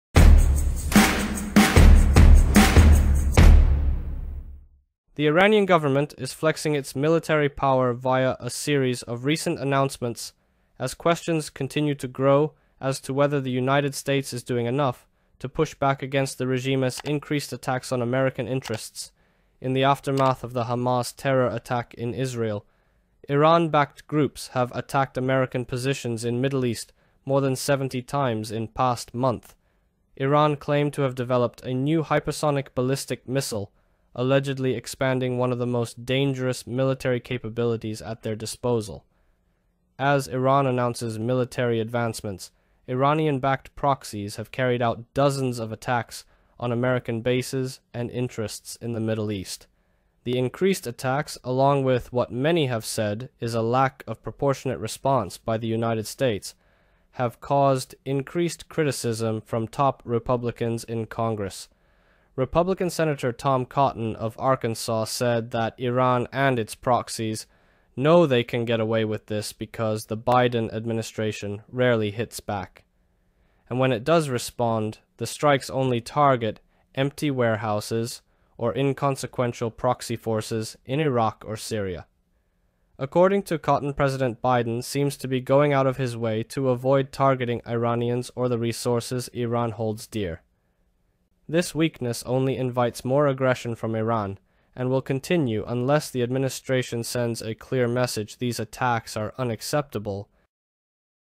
IRAN flexes military ADVANCEMENTS, increasing CALLS on BIDEN to ACT
The Iranian government is flexing its military power via a series of recent announcements as questions continue to grow as to whether the United States is doing enough to push back against the regime’s increased attacks on American interests in the aftermath of the Hamas terror attack in Israel.

Iran-backed groups have attacked American positions in Middle East more than 70 times in past month.